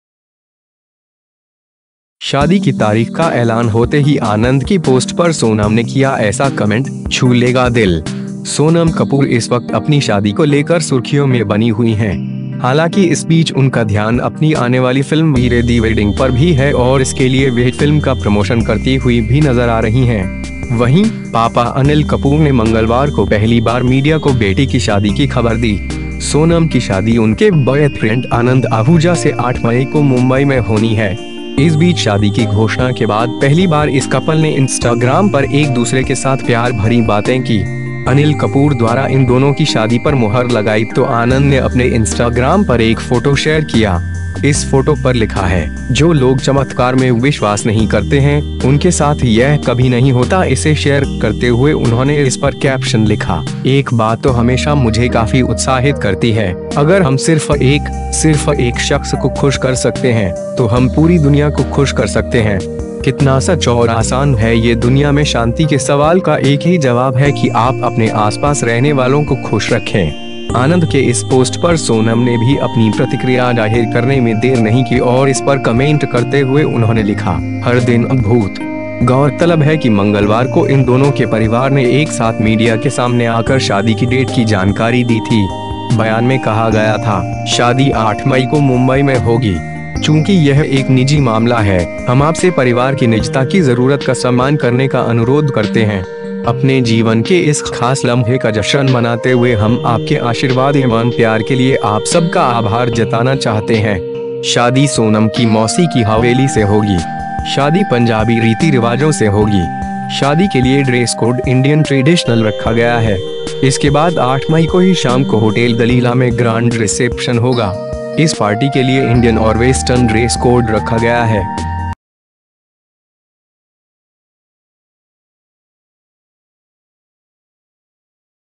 शादी की तारीख का ऐलान होते ही आनंद की पोस्ट पर सोनम ने किया ऐसा कमेंट, छू लेगा दिल
विराट कोहली के तोहफे को 'नुष्की' ने ऐसे किया कबूल, युवराज ने भी खास नाम से पुकारा

विराट कोहली ने जो वादा किया उसे ईमानदारी से निभाया। मंगलवार को अनुष्का शर्मा के बर्थडे के मौके पर उन्होंने आईपीएल में अपनी जीत को पत्नी को डेडिकेट किया।  मैच खत्म होने के बाद प्रेजेंटेशन सेरेमनी के दौरान विराट ने कहा, 'मेरी पत्नी यहां हैं और आज उनका बर्थडे है, इसलिए मैं खुश हूं कि उन्होंने हमें जीतते हुए देखा। यह उनके लिए एक छोटा सा तोहफा है। उनके सामने दो अंक हासिल करके बहुत खुशी मिली।'

विराट कोहली जब इस तरह अनुष्का से अपने प्यार का इजहार कर रहे थे, तो एक्ट्रेस का चेहरा देखने लायक था। विराट लगातार अनुष्का की तरफ देख रहे थे और उनकी 'नुष्की'  शर्म से लाल हो गई थीं। उनके चेहरे पर पूरे वक्त एक प्यारी से स्माइल खिली रही।

वहीं, क्रिकेटर युवराज सिंह ने भी अनुष्का शर्मा को खास नाम से संबोधित करते हुए जन्मदिन की बधाई दी। युवी ने एक्ट्रेस को 'रोजी भाभी' कहकर जन्मदिन की बधाई दी। बता दें कि फिल्म बॉम्बे वेल्वेट में अनुष्का के कैरेक्टर का नाम रोजी था। इसलिए युवी भी उन्हें रोजी भाभी कहकर पुकारते हैं।

विराट कोहली ने शादी के बाद अनुष्का शर्मा के पहले बर्थडे को यादगार बनाने में कोई कसर नहीं छोड़ी। मैच से पहले उन्होंने पूरा वक्त एक्ट्रेस के साथ बिताया। दिन में दोनों ने साथ मॉल जाकर फिल्म देखी। इसके बाद लंच पर भी गए। कोहली ने सोशल मीडिया पर अपनी और अनुष्का की एक फोटो भी पोस्ट की जिसमें वे दोनों एक दूसरे को केक खिलाते नजर आ रहे हैं।